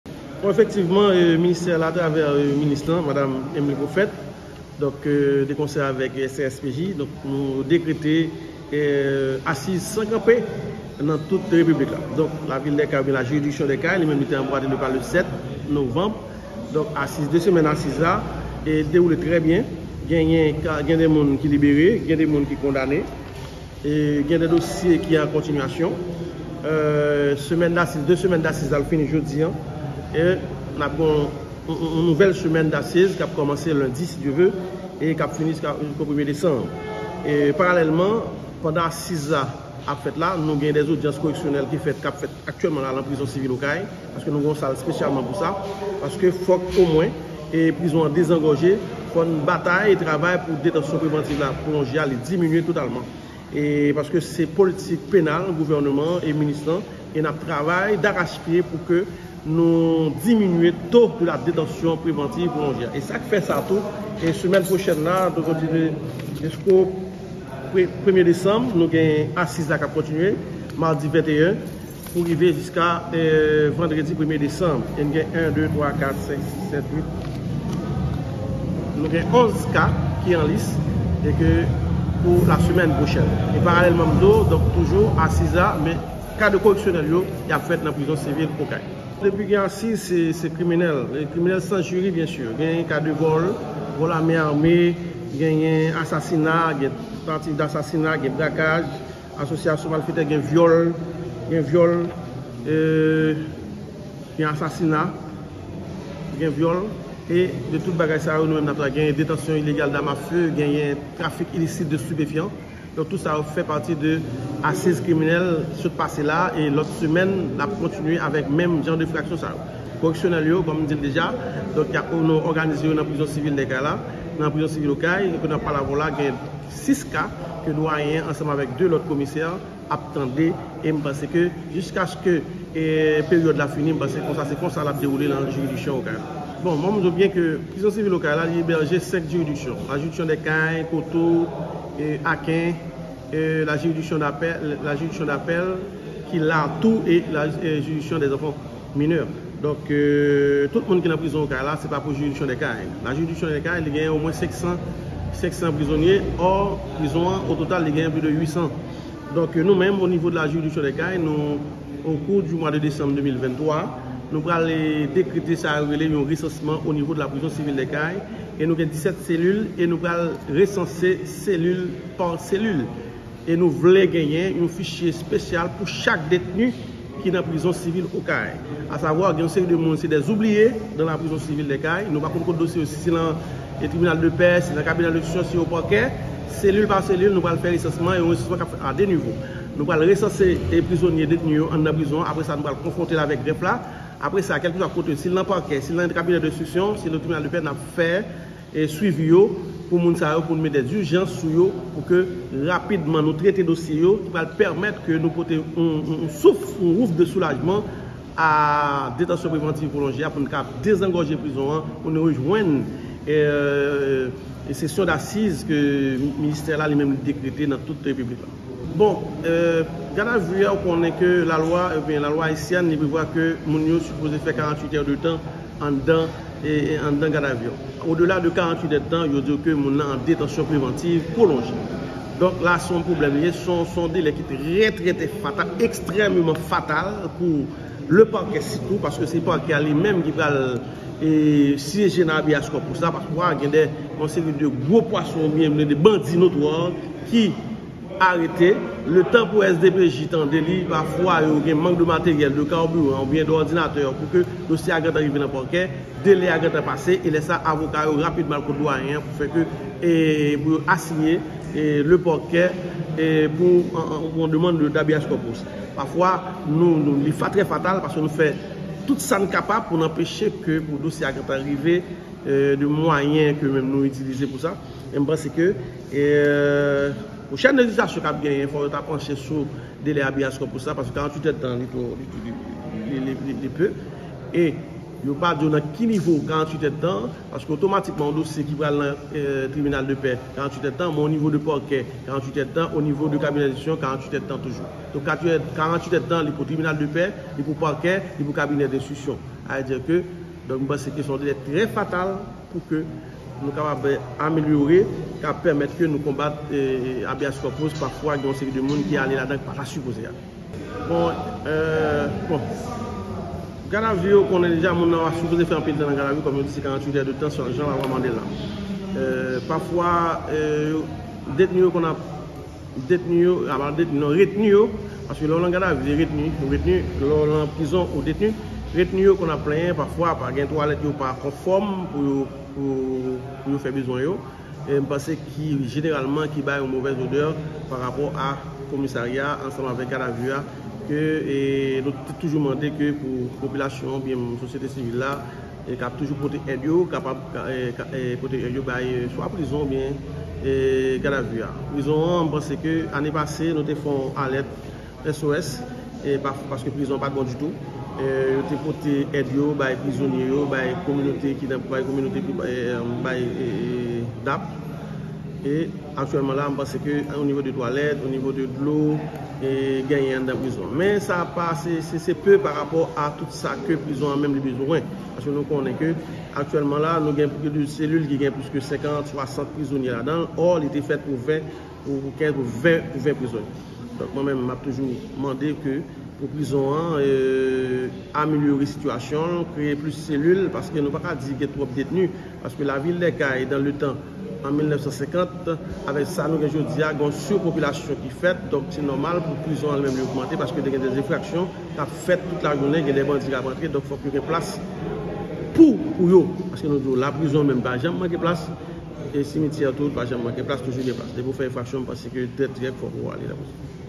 Komisè Ronald Richemond, prezante you bilan pozitif sou 2 semèn asiz kriminèl ak odyans koreksyonèl
Komisè gouvènman, Ronald Richemond, prezante you bilan pozitif sou 2 semèn asiz kriminèl ak odyans koreksyonèl ki fèt nan jiridiksyon Okay la soti 7 pou rive 17 novanm pase a. Chef pakè a deklare asiz San kanpe pou kapab libère prizon sivil Okay la ki gen plis pase 800 prizonye ladanl.
Chèf pakè a anonse pral mete sou pye yon resansman pou kapab idantifye you seri prizonye ke yo bliye epi travay rapid sou dosye yo, toujou man lojik pou reponn ak politik gouvènman ki lanse yon konba nan tout peyi a kont detansyon prevantiv pwolonje.